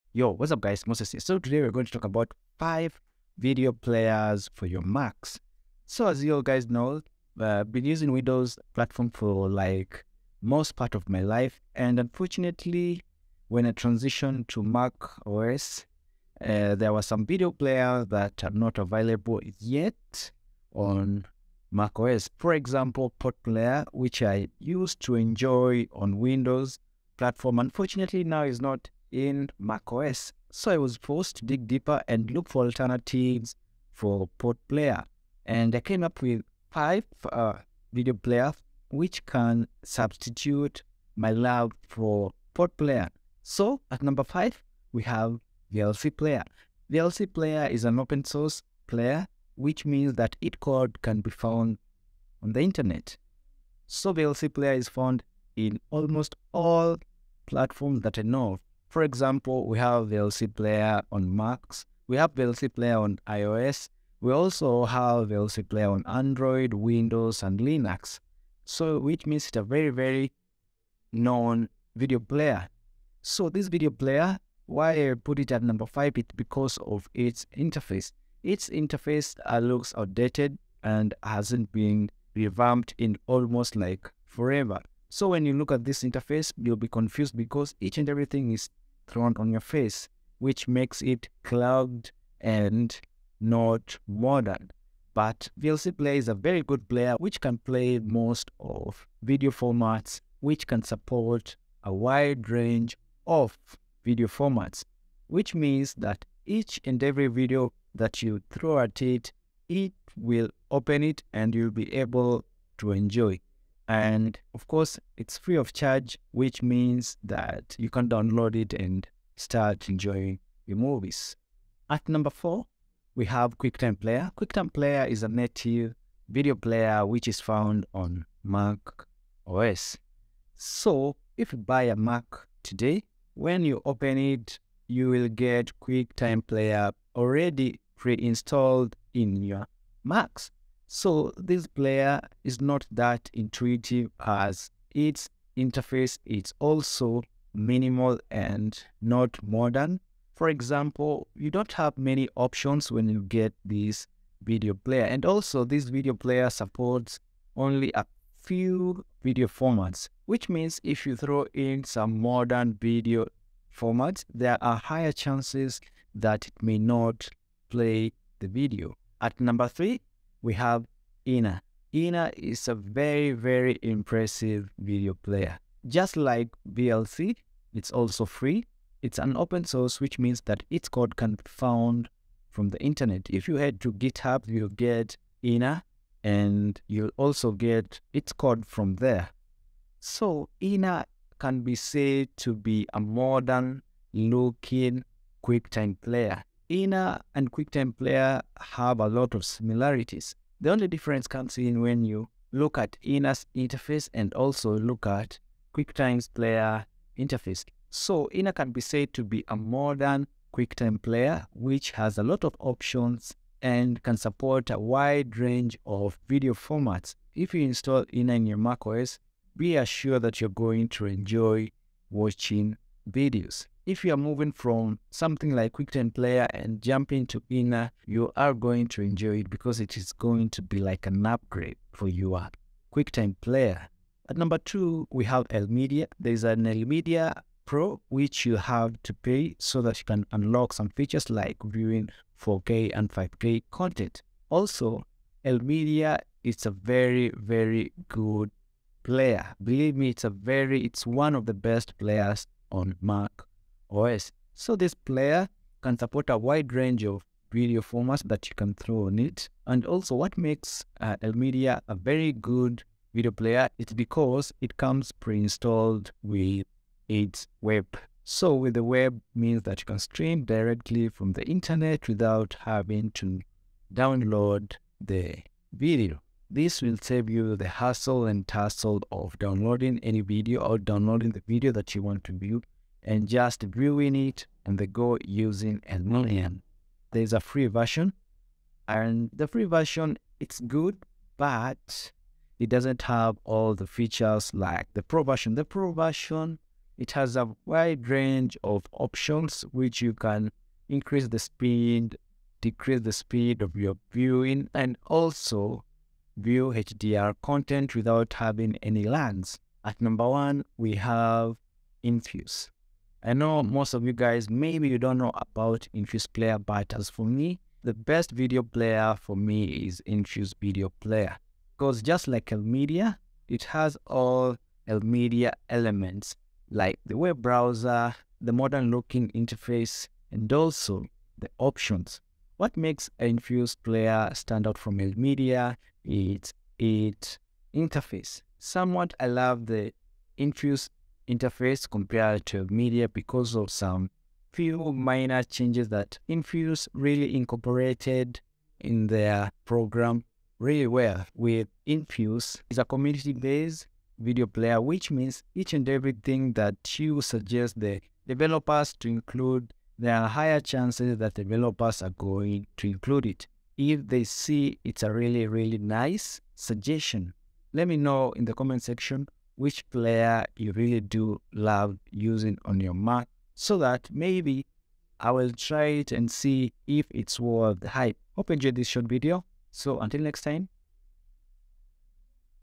Why Mac Users Love These Video Players in 2023
These are some of the video players that I have been using. I know there are a lot that I haven't mentioned please feel free to add on the comment section some of your best Video player for MacBook.